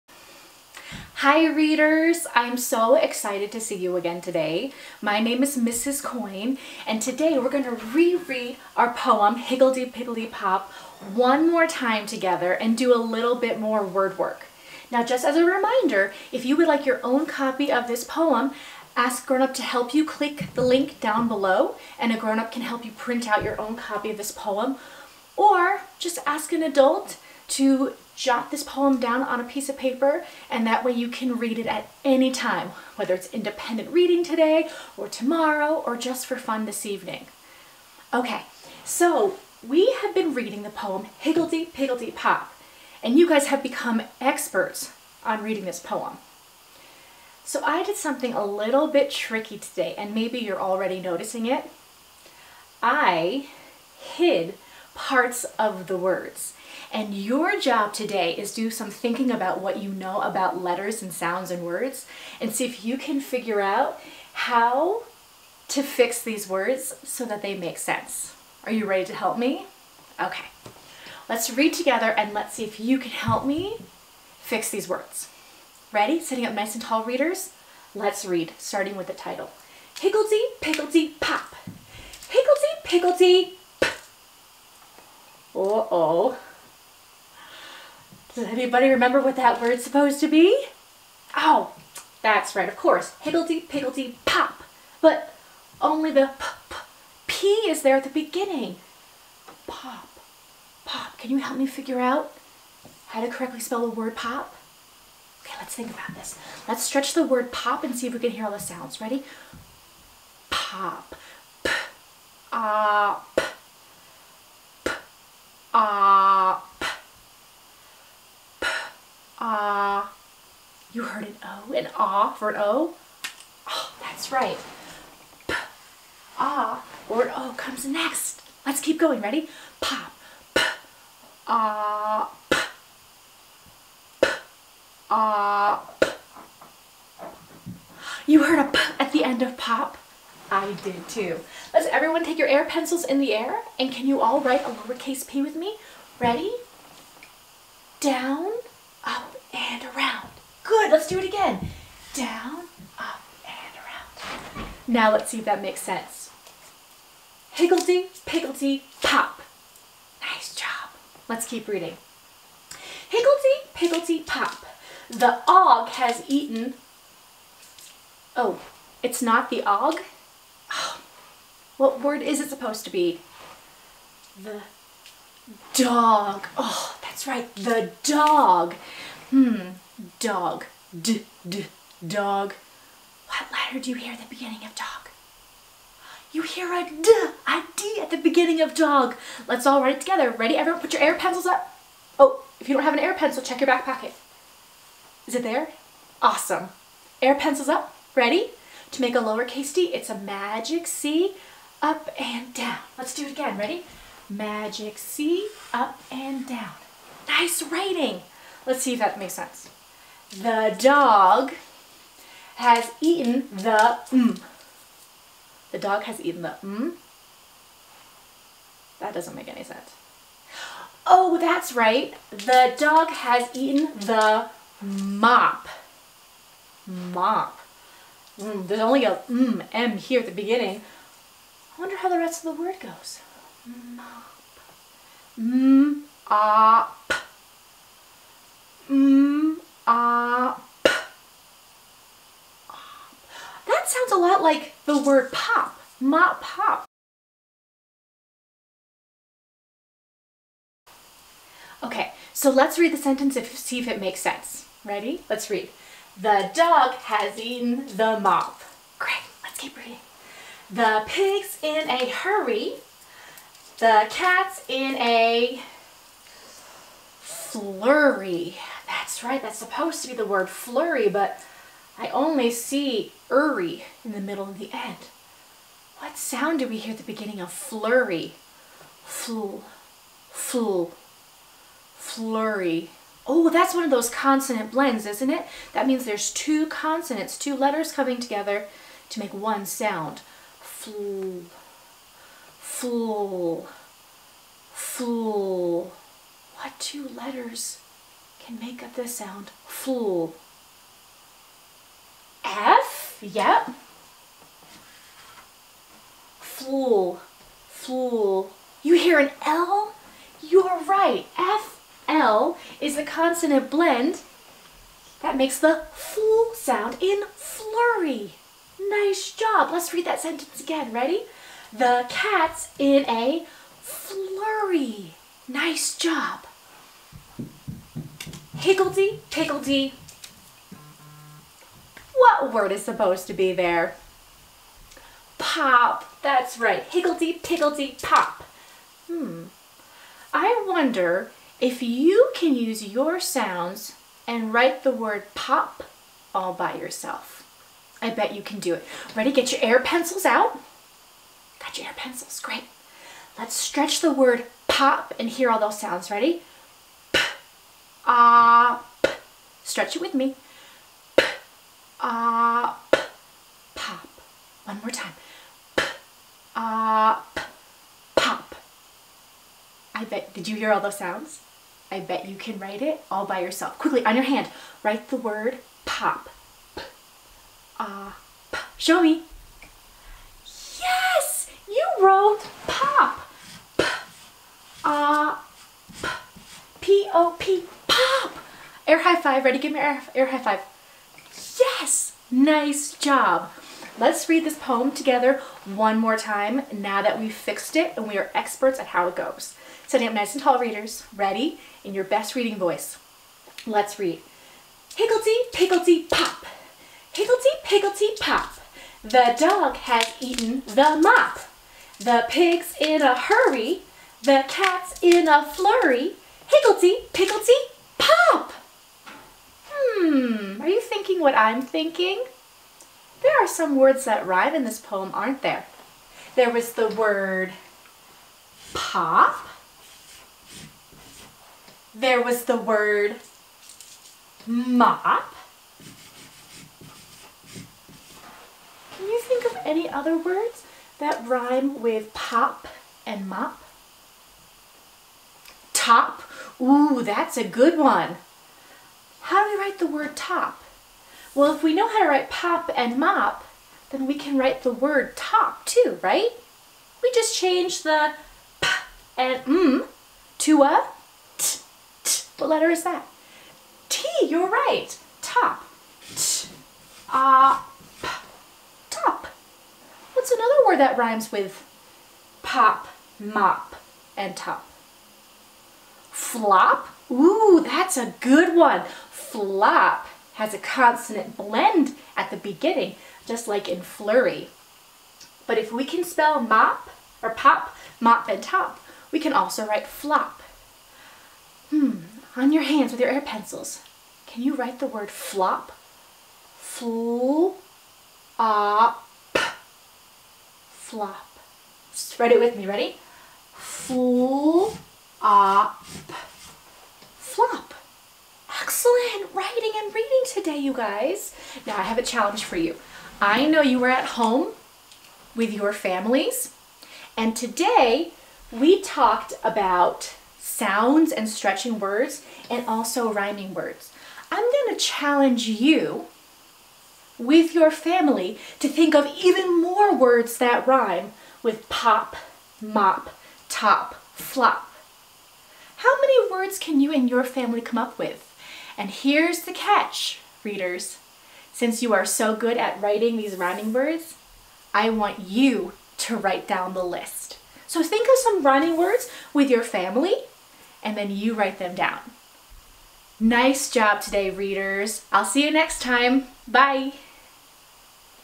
Higglety Pigglety Pop! Shared Reading Day 3
Note: the camera card got full in the middle of writing the word "mop" and cut off the spelling of it together, so I inserted the correct spelling of "mop" during editing.